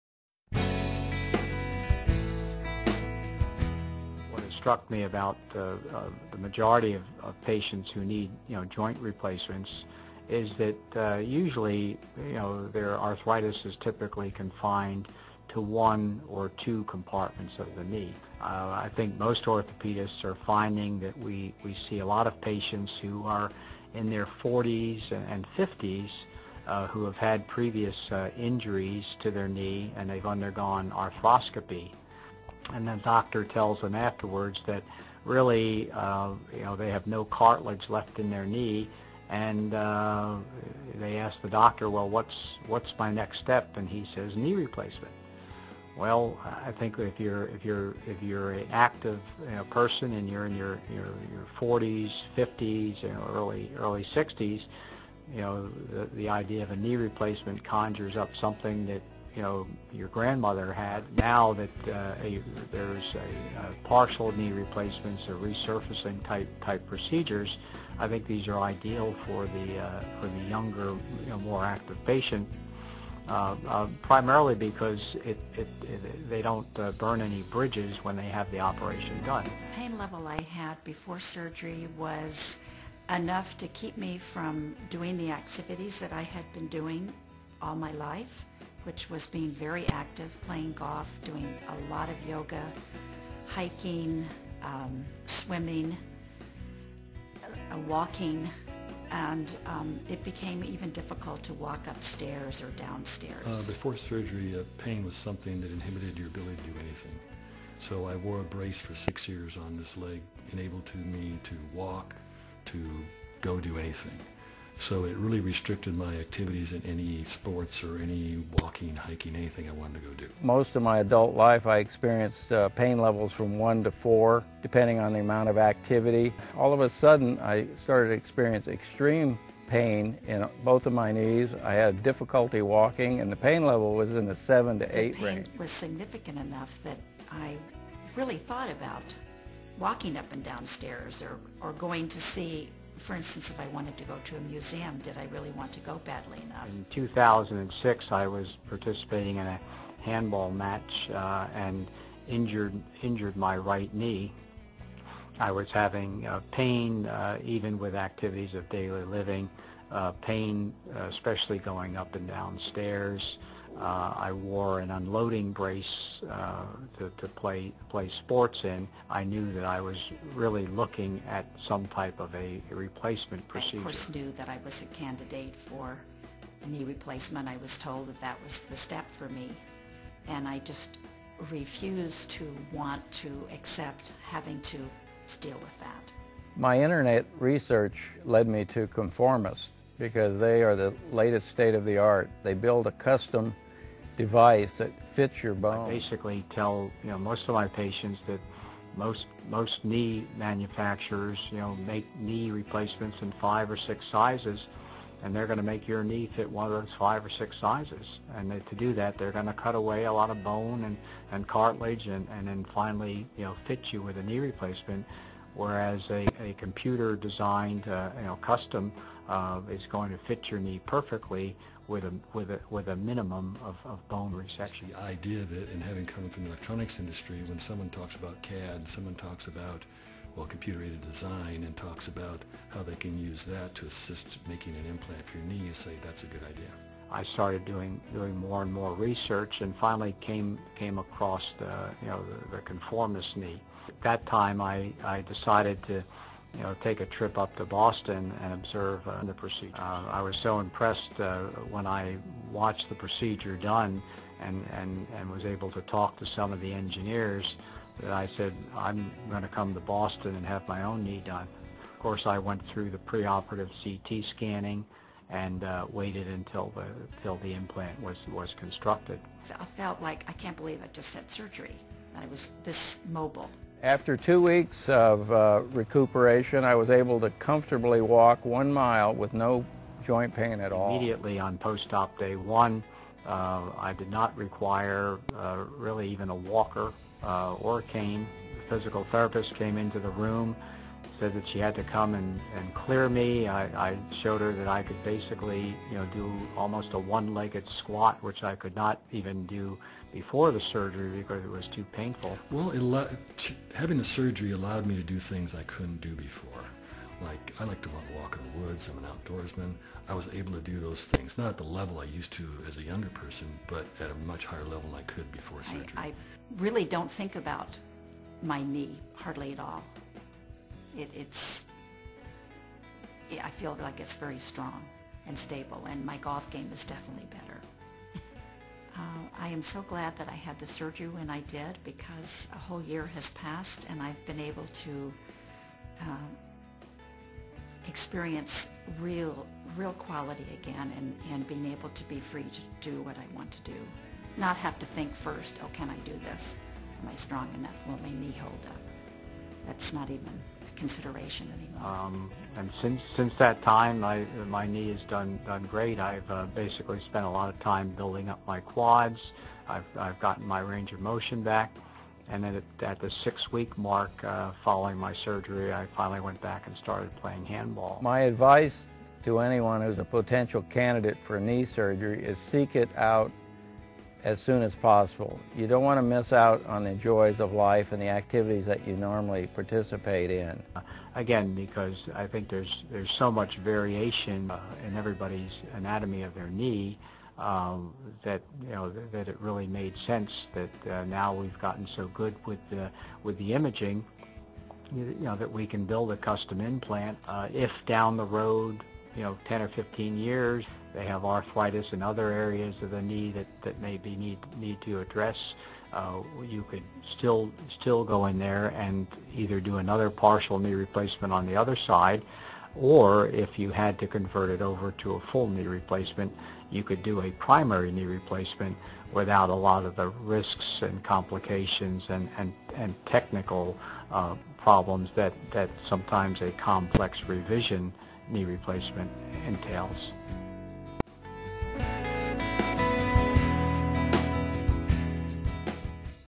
Personalized Stories
Patients share their personal stories about their experience with ConforMIS knee implants.